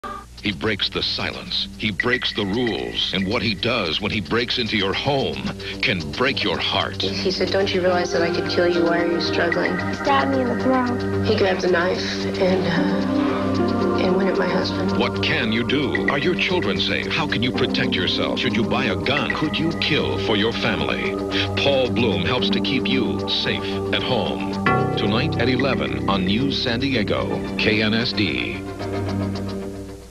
KNSD News San Diego promo, 1991
Promo for an upcoming special report on staying "safe at home".